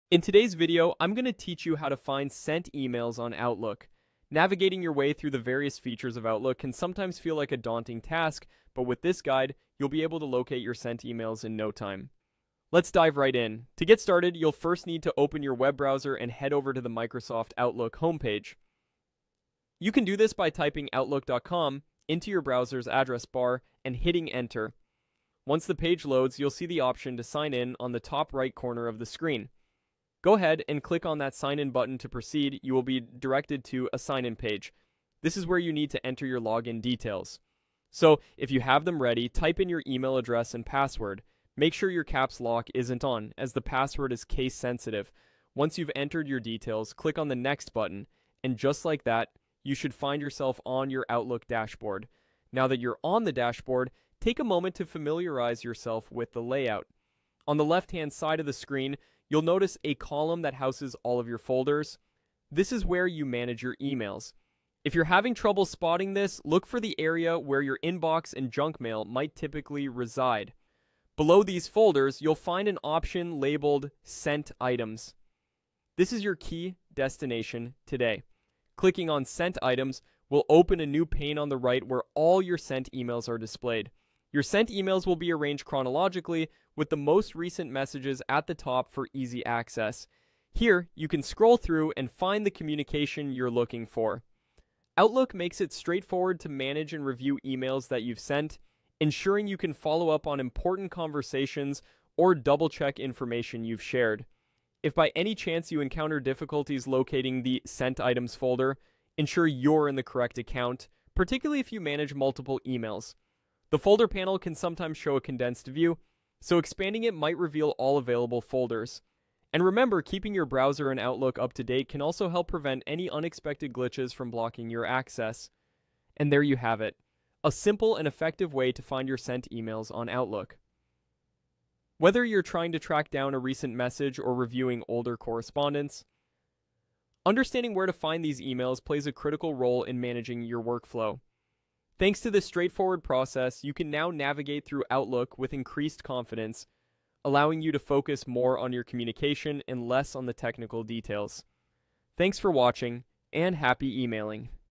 How to Find Sent Emails on Outlook (Easiest Way) (2026 Guide)
Struggling to locate your sent emails in Outlook? This step-by-step guide shows you exactly how to find sent messages in Outlook on Desktop, Web (Outlook on the Web/Outlook 365), and the Mobile app. Whether you're tracking communication or need a record for reference, this tutorial has you covered!

Timestamps:
00:00 - Introduction
00:36 - Accessing Sent Emails in Outlook Desktop
02:05 - Locating Sent Items in Outlook Web (365)
03:40 - Using Search Function to Find Sent Emails
05:10 - Checking Sent Items on Outlook Mobile App
06:30 - Troubleshooting Missing Sent Folder
07:50 - Bonus Tips for Organizing Sent Emails

Features covered: Sent Items folder navigation, advanced search, troubleshooting tips, supported across Outlook 365, Desktop, and Web platforms.